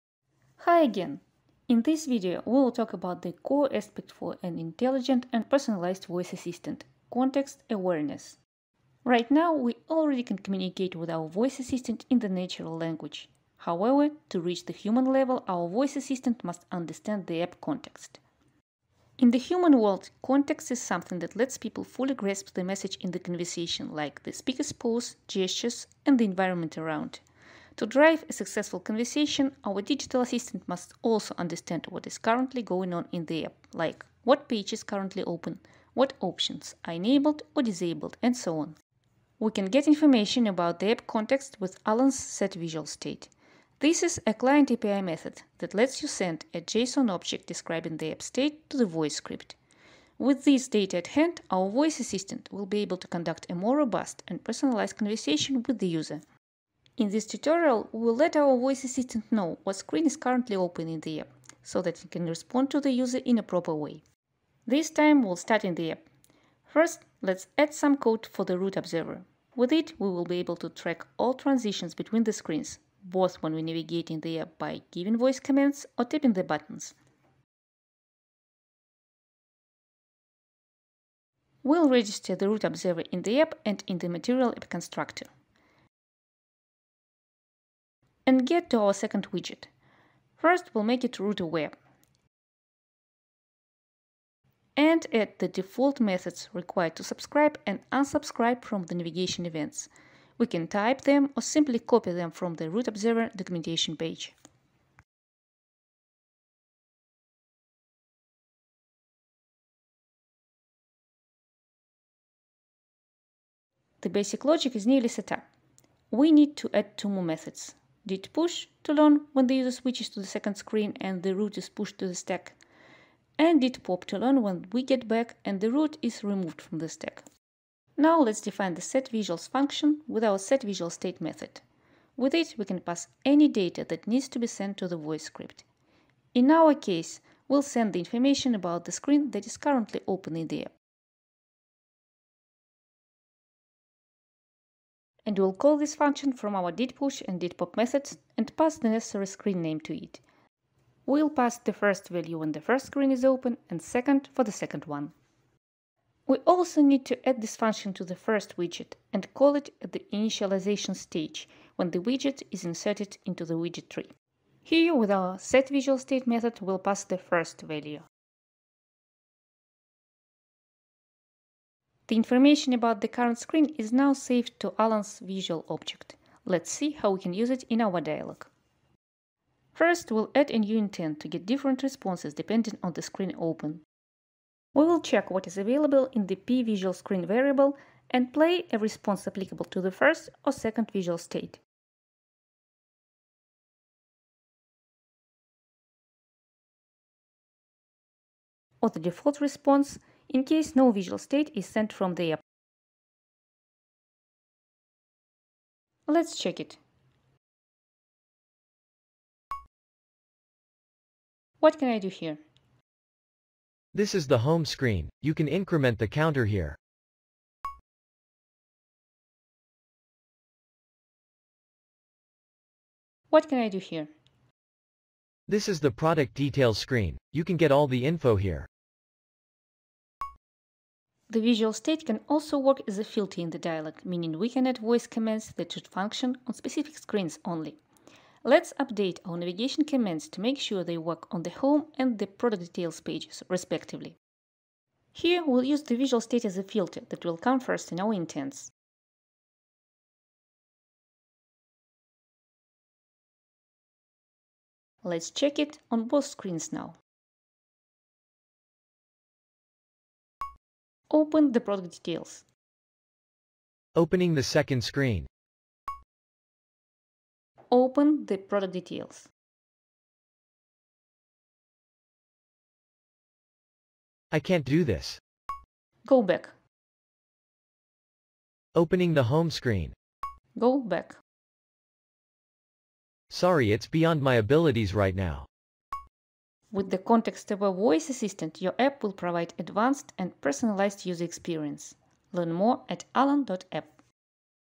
Creating a Context Aware Voice Assistant
With Alan AI, you can design a truly intelligent digital assistant being able to complete any tasks with voice. To mimic the human interactions, your voice assistant must understand what is currently going on in the app. Learn how you can capture the current visual context of a Flutter app and use this information to adjust your dialog.

Video guide:
Intro: (00:00)
Adding a RouteObserver (01:10)
Adding an Intent to the Voice Script (02:58)
Filtering Voice Commands (03:53)